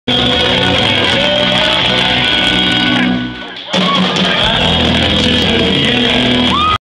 Jamie Moses & Mark Hudson
Jamie Moses & Mark Hudson sing while playing guitar